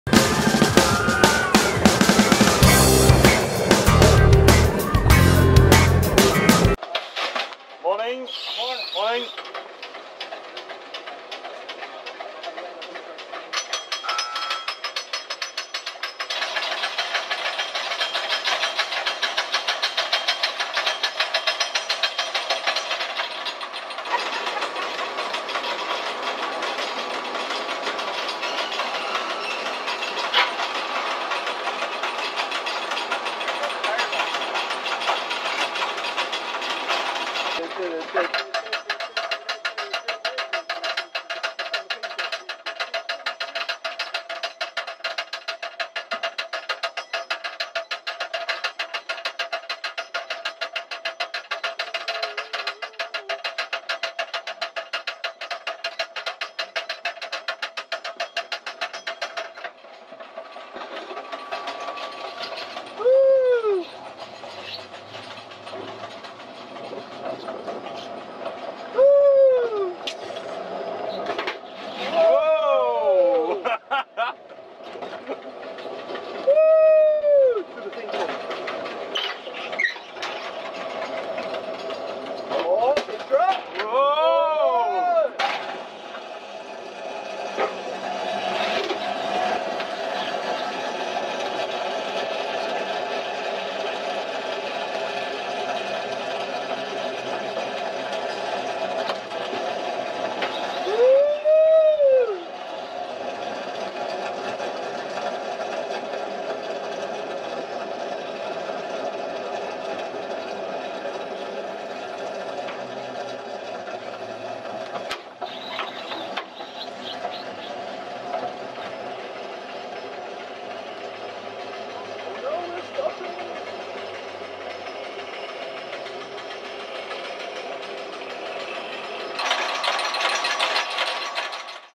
Slope shooter
On ride footage of the Slope shooter bizare roller coaster at Higashiyama Zoo. Shot in 2005